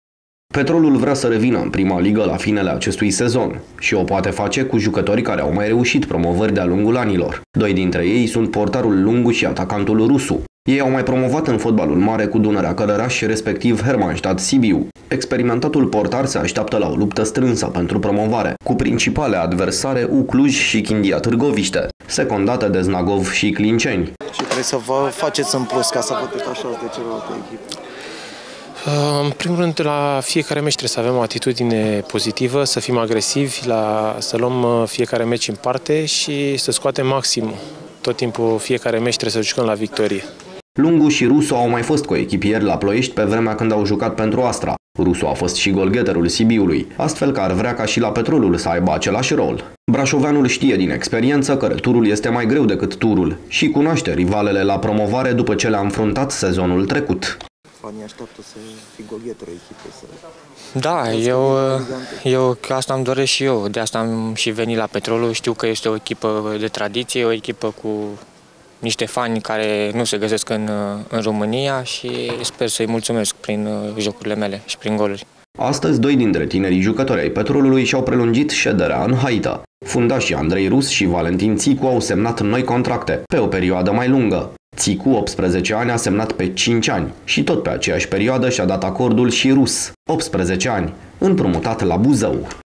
Vor să bifeze o nouă promovare!
Petrolul vrea să revină în prima ligă la finalul acestui sezon. Şi o poate face cu jucători care au mai reuşit promovări de a lungul anilor. Doi dintre ei sunt portarul Lungu şi atacantul Rusu. Ei au mai promovat în fotbalul mare cu Dunărea Călăraşi respectiv Hermannstadt Sibiu.  Experimentatul portar se aşteaptă la o luptă strânsă pentru promovare cu principale adversare U Cluj şi Chindia Târgoviște, secondate de Snagov şi Clinceni.
Lungu şi Rusu au mai fost coechipieri la Ploieşti pe vremea când au jucat pentru Astra. Rusu a fost şi golgheterul Sibiului astfel că îşi doreşte şi la Petrolul să aibă acelaşi rol. Braşoveanul ştie, din experienţă, că returul este mai greu decât turul şi cunoaşte rivalele la promovare după ce le-a înfruntat sezonul trecut.
Astăzi, doi dintre tinerii jucători ai Petrolului, și-au prelungit șederea în haită. Fundașii Andrei Rus și Valentin Țicu au semnat noi contracte de joc pe o perioadă mai lungă. Țicu, 18 ani, a semnat pe cinci ani de zile și tot pe aceeași perioadă și-a dat acordul și Rus, 18 ani, împrumutat la Buzău.